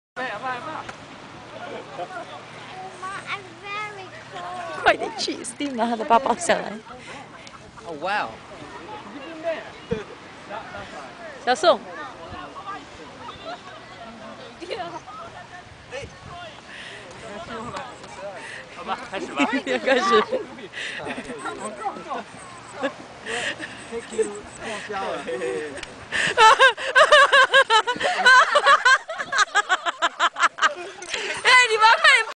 James ambush.wmv
Watch as James gets caught in Chen Rongs trap. CR pretends to ask a question of James. Then while James explains, Steve creeps up and craftily undoes his bag, and then acts all surprised when it falls off. Then to further distract James, CR starts feeling James biceps and saying how strong he is. James by this time seems to smell a rat but doesn't quite know what to do. Then the others get into position to lift him up and take him down to the sea to chuck him in. Watch James sudden realisation and subsequent violent struggle. I finish the video early because James actually hurts himself a little bit so I've cut that bit out. Still very funny though.